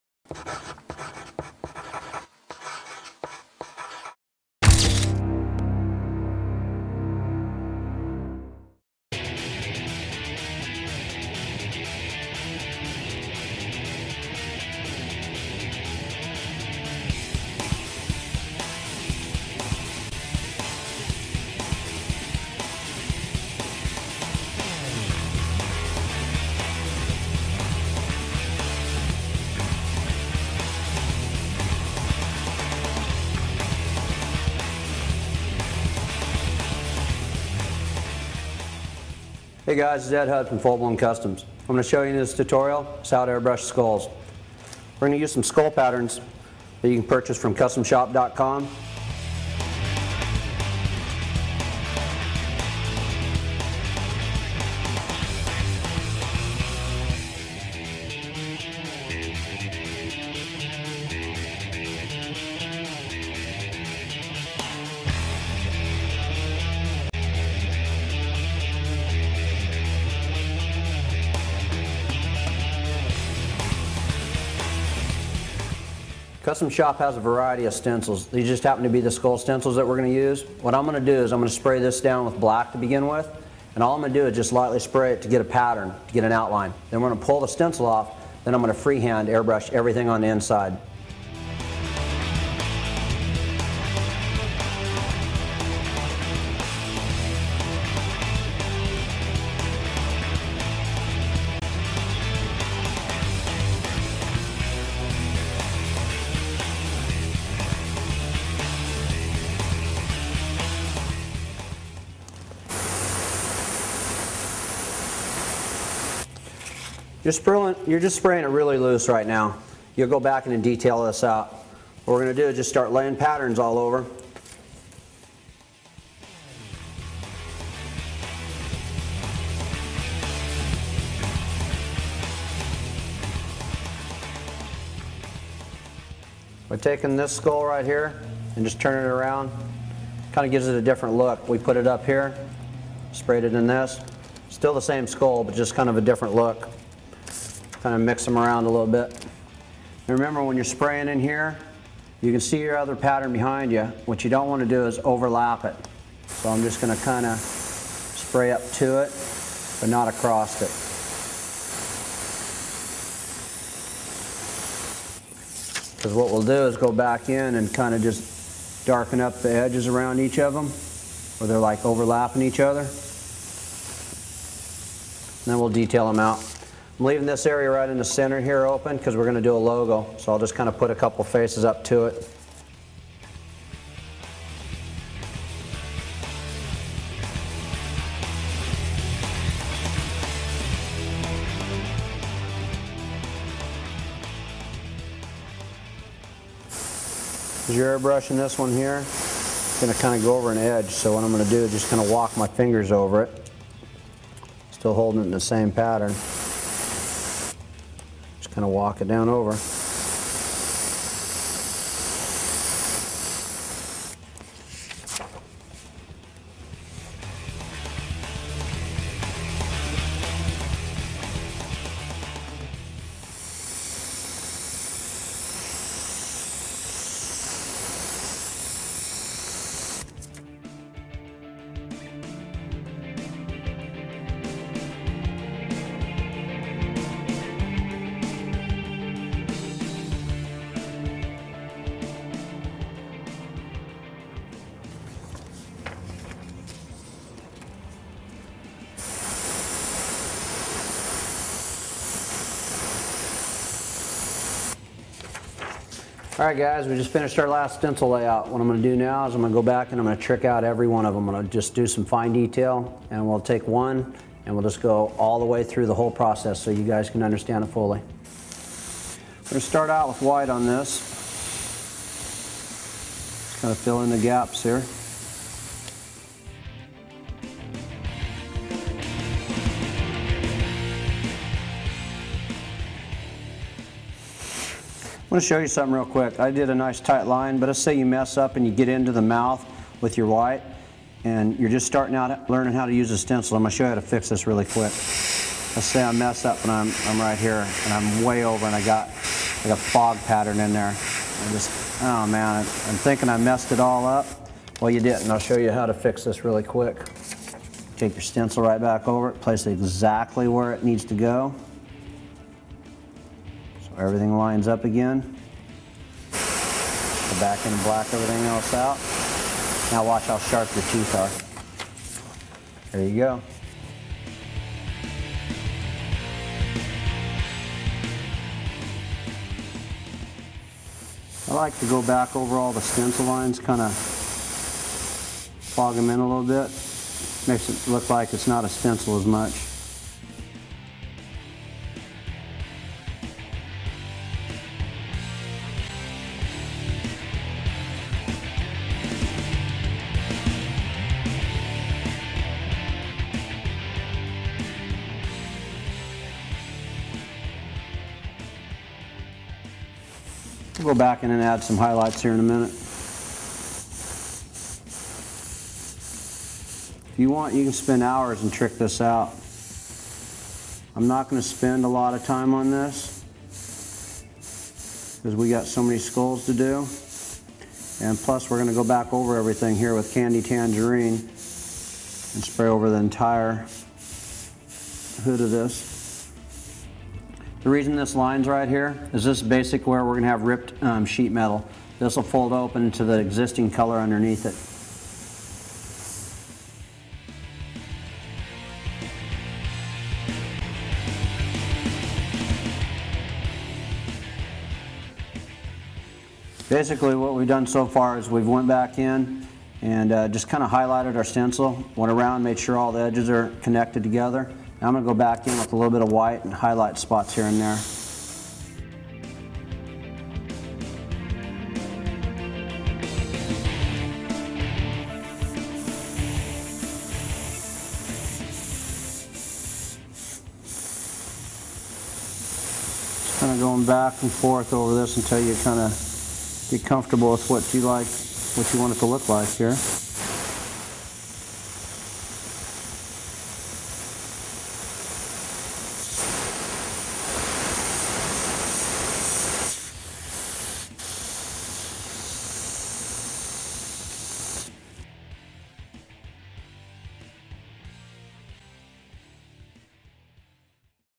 Airbrushing SKULLS - [part - 1 of 2]
In this tutorial Ed teaches you how to layout multiple skulls using different stencil patterns to create an amazing layering effect.